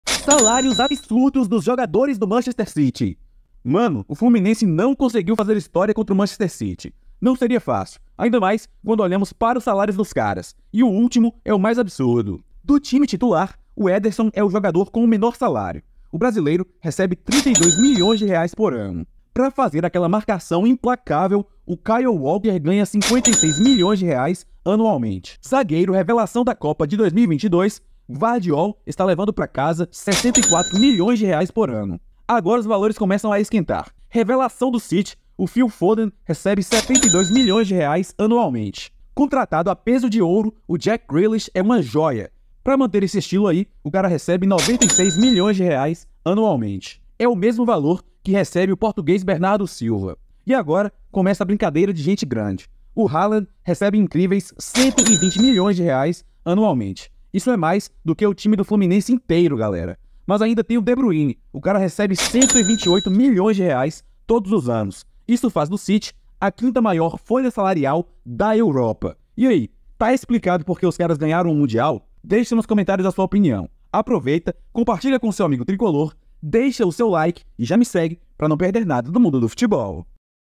SALÁRIOS ABSURDOS DOS JOGADORES DO MANCHESTER CITY
O vídeo destaca os salários astronômicos dos jogadores do Manchester City após a partida contra o Fluminense. O apresentador menciona que o Fluminense enfrentou dificuldades diante do City, especialmente ao considerar os salários dos jogadores da equipe inglesa. O vídeo destaca alguns salários impressionantes: Ederson com o menor salário do time titular, recebendo 32 milhões de reais por ano; Kyle Walker com 56 milhões anualmente; Gvardiol, revelação da Copa de 2022, com 64 milhões por ano; Phil Foden com 72 milhões; Jack Grealish e Bernardo Silva com 96 milhões cada; Haaland com incríveis 120 milhões por ano; e Kevin De Bruyne liderando a lista com 128 milhões, fazendo do City a 5ª maior folha salarial da Europa. O vídeo sugere uma relação entre os salários e o desempenho do time no mundial, convidando os espectadores a deixarem suas opiniões nos comentários, compartilharem com amigos tricolores, deixarem likes e seguirem o canal para não perderem nada do mundo do futebol.